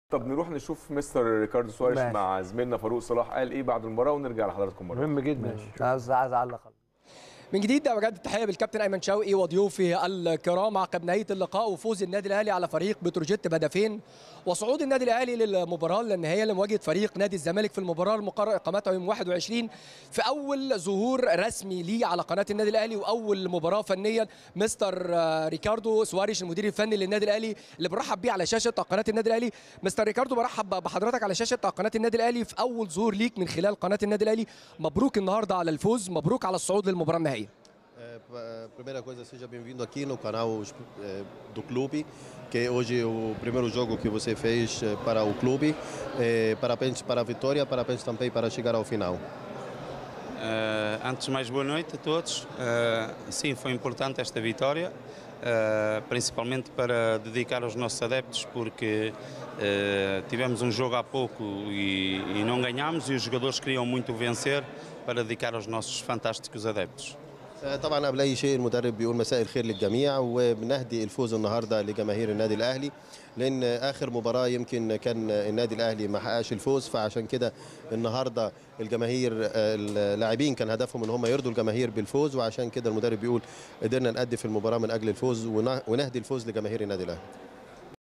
ريكاردو سواريش: بهدي فوزي الأول مع النادي الأهلي للجماهير.. وسعيد بظهوري على شاشة قناة الأهلي.
ريكاردو سواريش: بهدي فوزي الأول مع النادي الأهلي للجماهير.. وسعيد بظهوري على شاشة قناة الأهلي | الاستوديو التحليلي